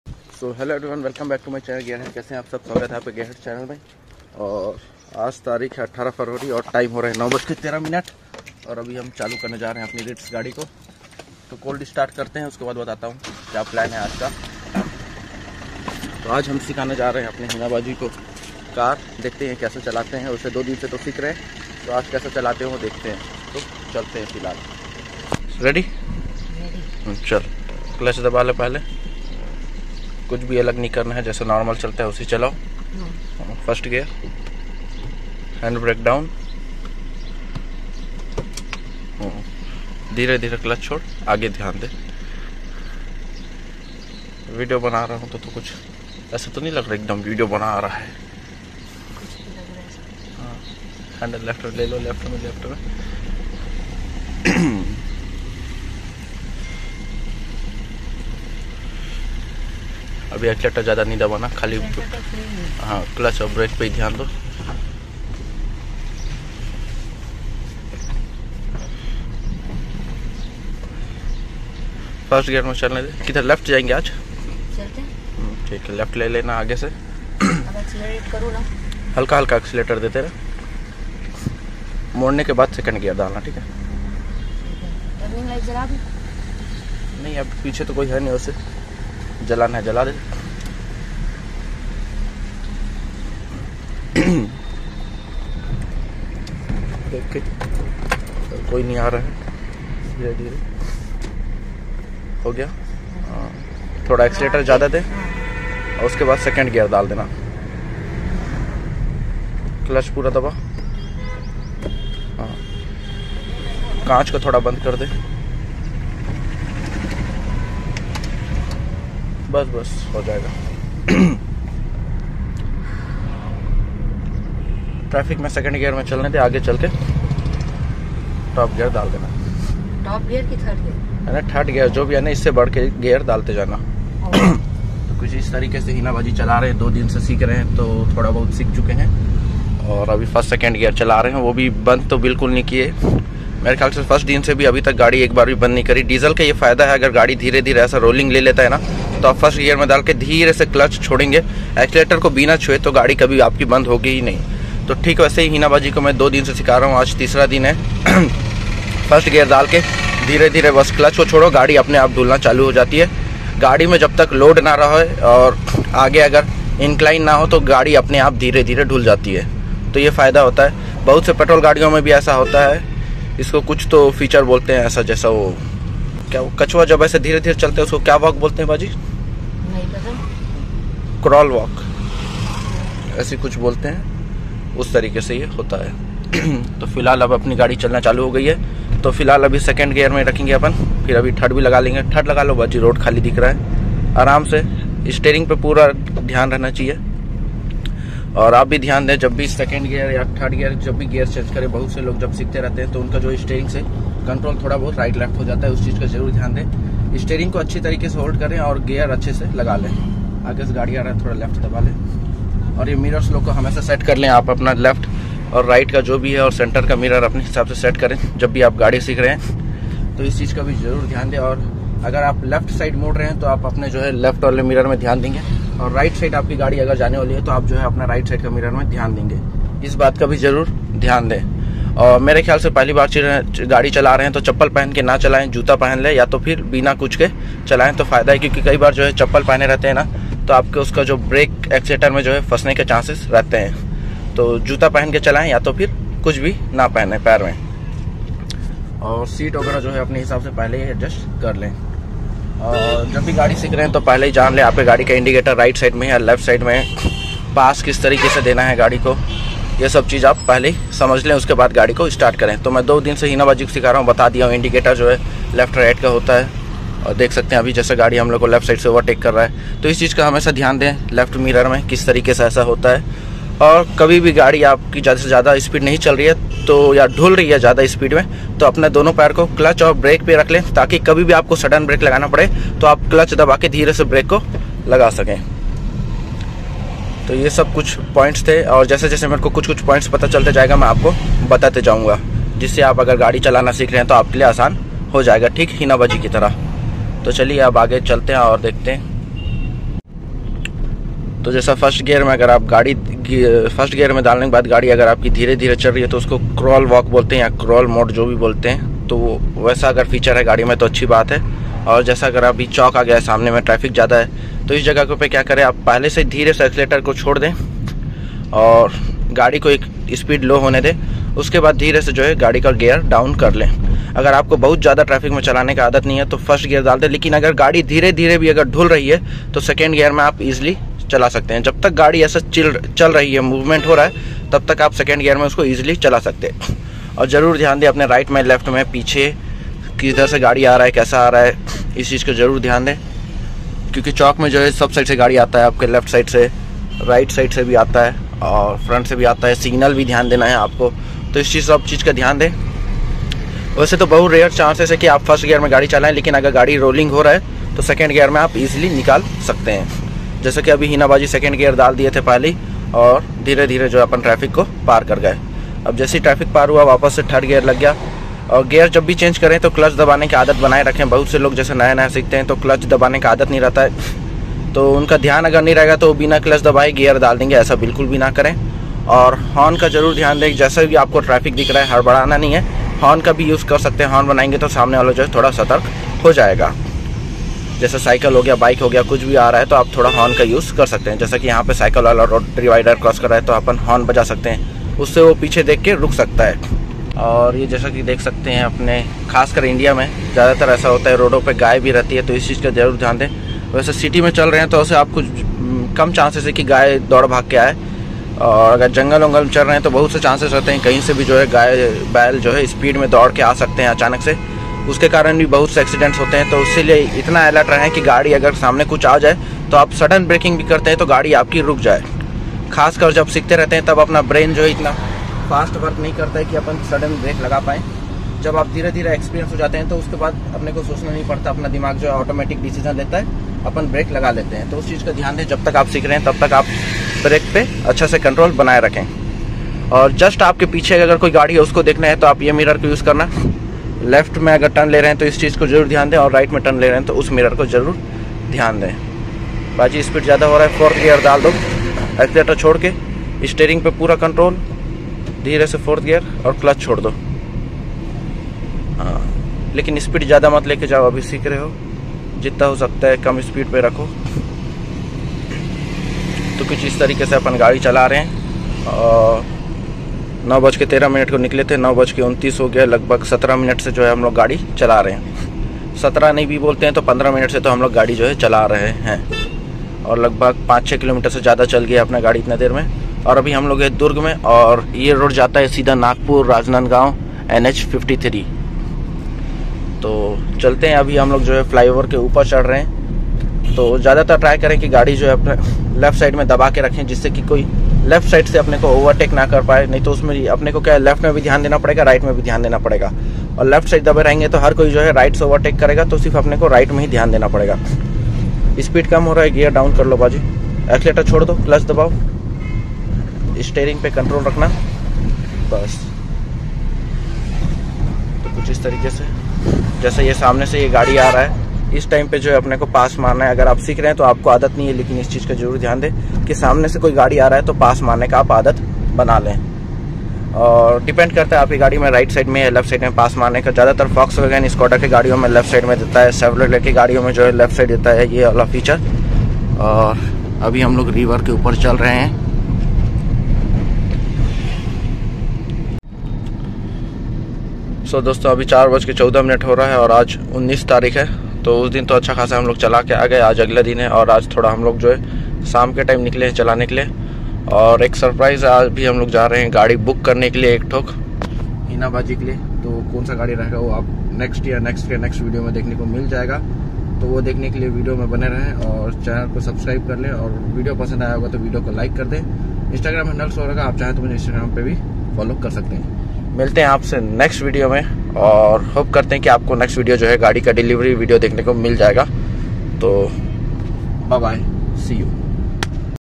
Teaching My Sister To drive a Car | Diesel Ritz
Teaching my sister @HeenaSheikh to drive a manual transmission car
maruti suzuki ritz vdi